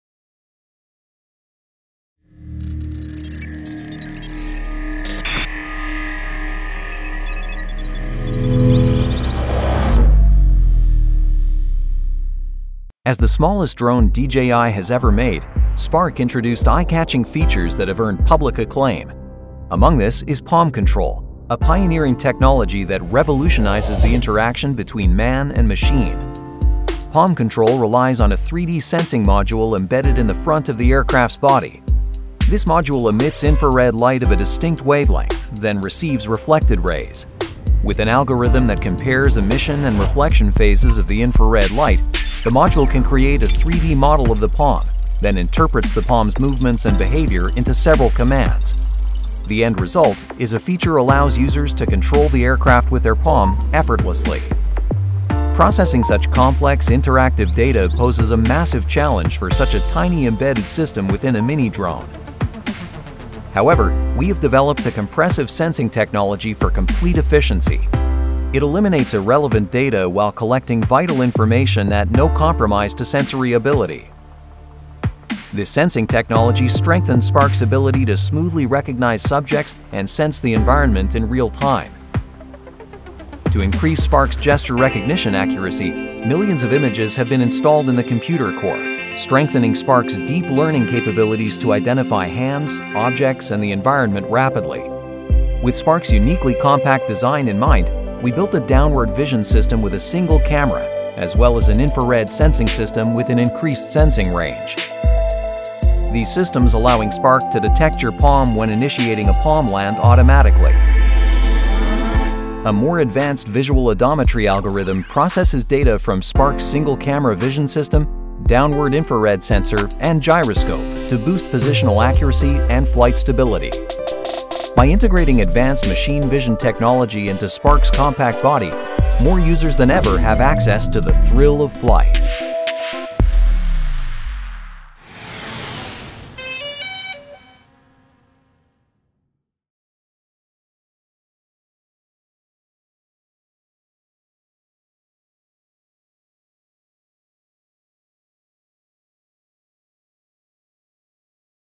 DJI — Technologies: PalmControl
DJI's smart mini-drone Spark introduced an entirely new dimension of drone technology and intelligence with the introduction of PalmControl. Users can tell Spark where to fly and when to take a photo or video with just a wave of their hand. The secret to this revolutionary feature is its advanced machine vision technology, which incorporates 3D sensing, infrared light, algorithmic analysis, and the power of deep learning into Spark's compact design. Watch to learn more about the intricate details of this drone's exceptional gesture recognition.